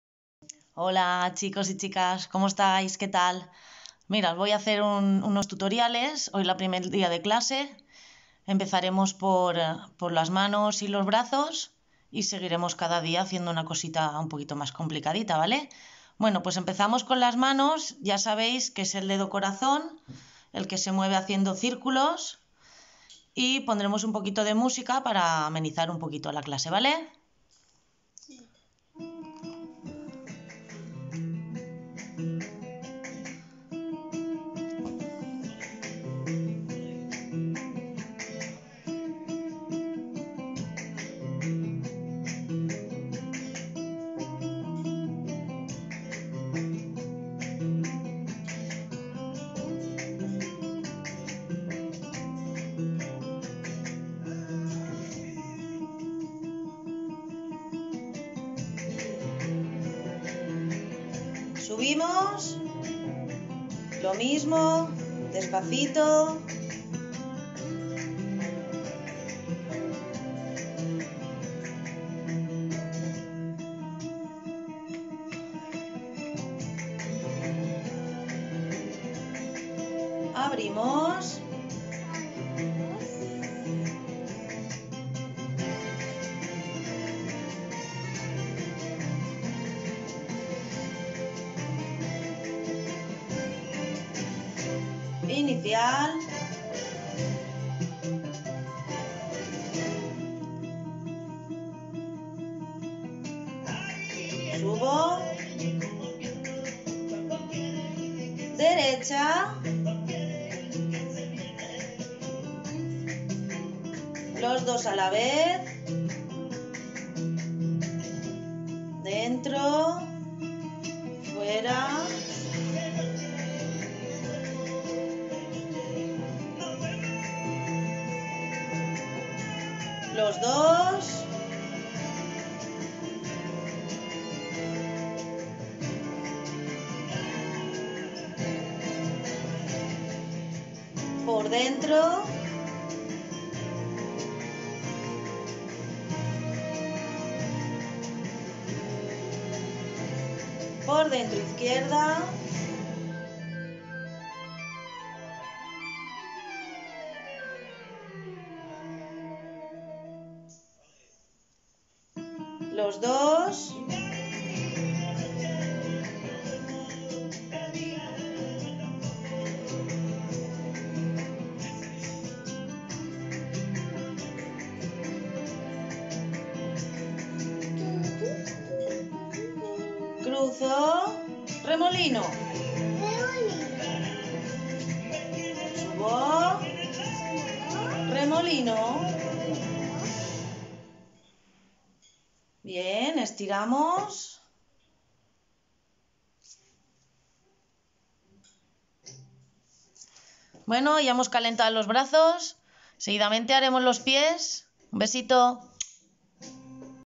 1 Sevillanas (CMC Roís de Corella y Corea). Ciutat Saludable Gandia. Impartidas por Alexandra Pla
Sevillanas. (CMC Roís de Corella y Corea).Por Alexandra Pla